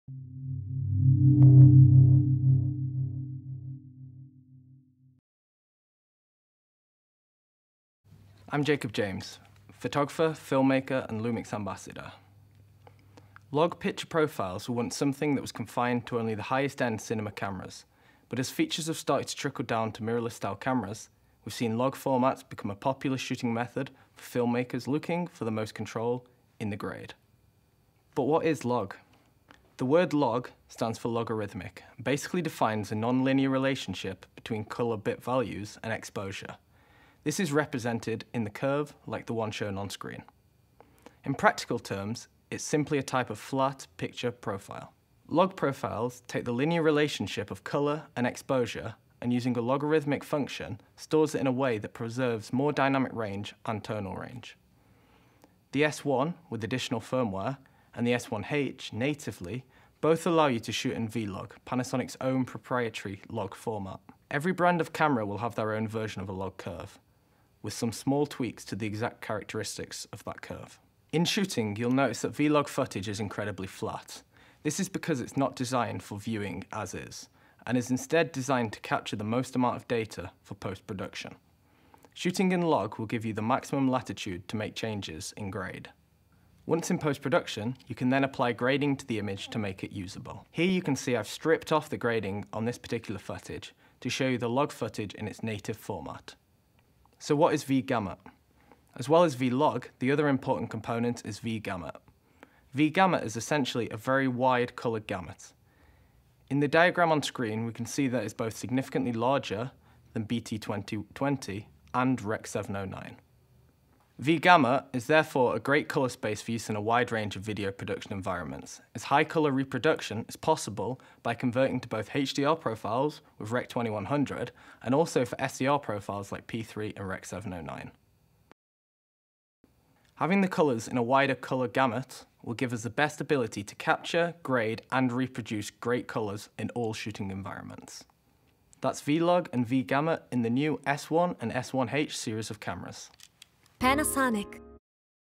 Panasonic - LUMIX S series - DC-S1H - Understanding V-LOG and V-GAMUT
V-Log is a logarithmic curve which describes the relationship between colour bit values and exposure. Using V-Log will preserve the most dynamic and tonal range from a RAW-signal making it the best format to prepare for post-production. LUMIX Ambassador Jacob James, introduces this format and the corresponding colour space: V-Gamut. Jacob shows you the relationship of V-Gamut to other colour profiles, such as Rec.709, Rec.2020 or Rec.2100, ensuring compatibility to your destination project format or colour space.